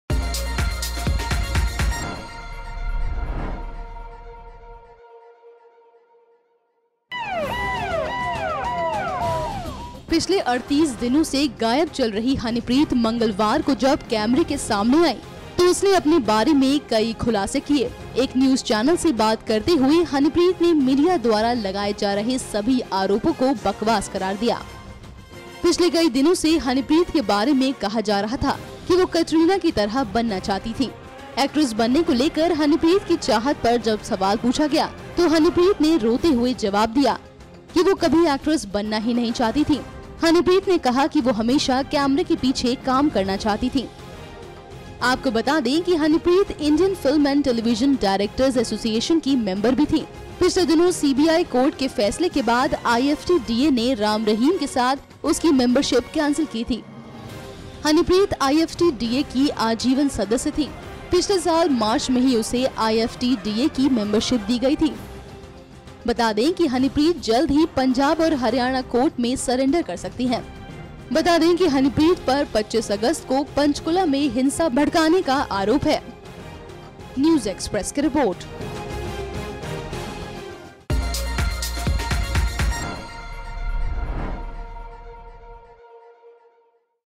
Honeypreet बोली- हमेशा Camera के पीछे रहकर ही करना चाहती थी काम
Honeypreet Insaan has said the allegations about her relationship with her adopted father Dera Sacha Sauda chief Gurmeet Ram Rahim Singh are baseless. In an interview Honeypreet said that she will surrender after legal advice. This came amid reports that she will surrender to the police by Wednesday.

producer - prakash
video editor - Vineet Singh